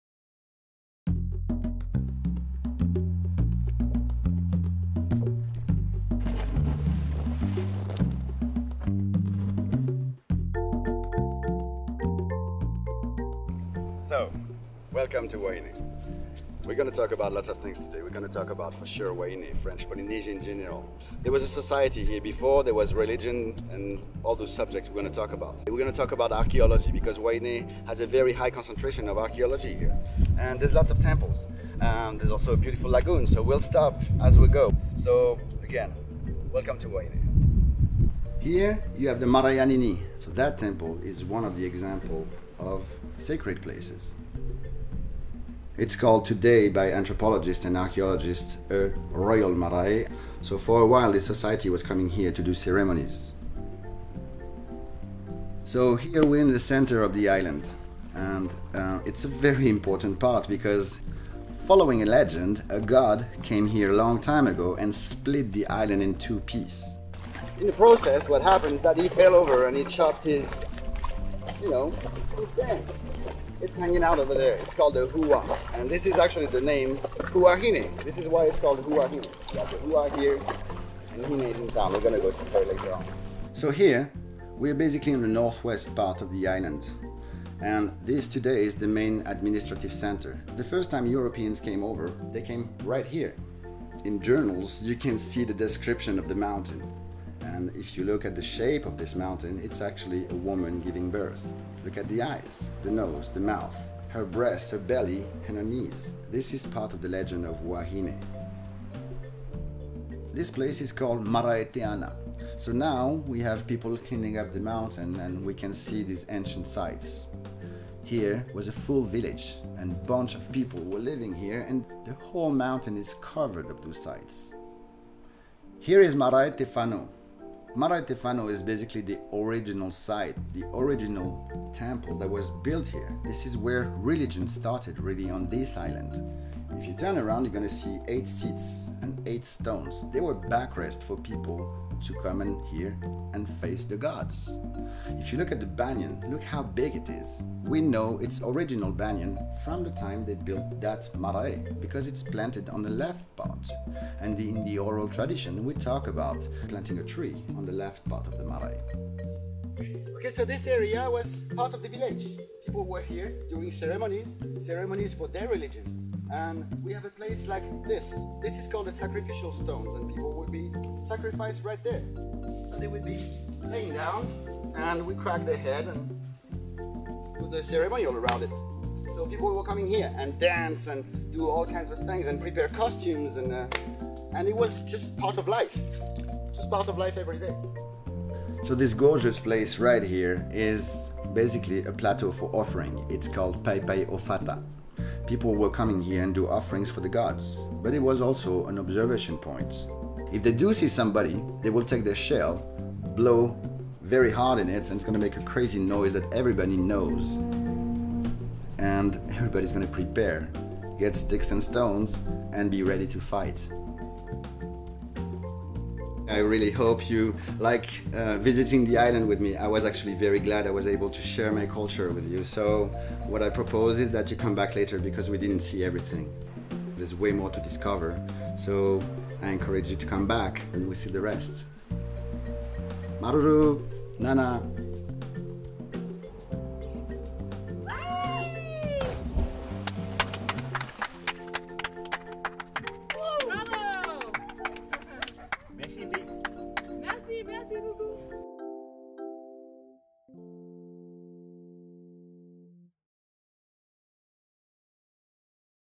Ancient Sites of Huahine Tahiti in VR 360 Video
Learn about the birth of religion, human sacrifice and folklore on Huahine, French Polynesia, with a local guide.

Huahine may not be a household name like Bora Bora or Tahiti—that’s because this sleepy island in the South Pacific is one of Polynesia’s best-kept secrets. Formally home to Polynesian royalty, Huahine has the highest concentration of  pre-European marae (sacred meeting places) in all of Polynesia.

See the ancient marae and learn about the ceremonies preformed here, visit the scenic shores where the Europeans first landed and find out where religion started on this island. Your local guide Manava with Island Eco Tours will share the culture of his ancestors with you, from tales of battles and human sacrifice to the folkloric birth of Huahine.

Note: This is an immersive 3D virtual reality experience. For better results, view in a Google Cardboard using the YouTube app on your mobile device, or in the Firefox, Chrome, Opera, or Internet Explorer desktop browser. For the best experience, please visit us on Samsung VR in your Gear VR headset at: